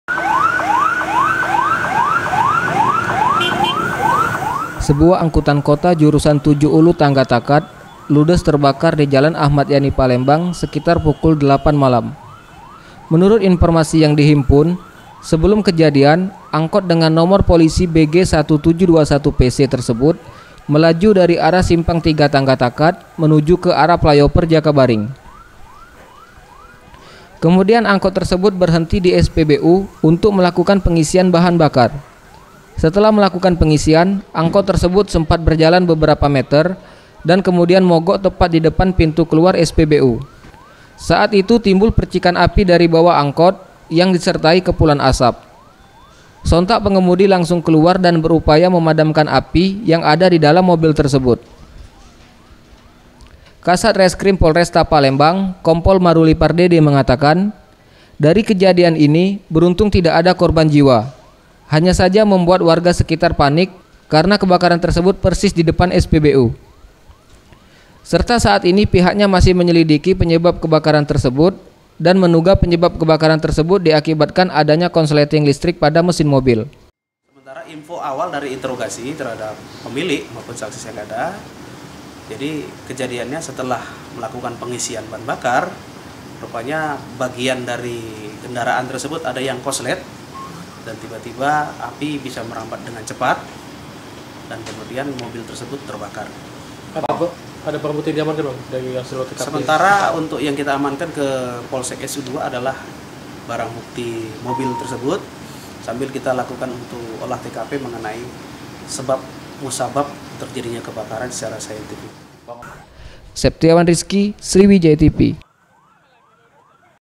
Angkot Jurusan 7 Ulu Ludes Terbakar
Kasus kebakaran mobil terjadi lagi. Kali ini menimpa angkutan kota jurusan 7 Ulu-Tangga Takat yang hangus terbakar usai melakukan pengisian bahan bakar. Dugaan sementara adanya konsleting listrik yang menyebabkan percikan api.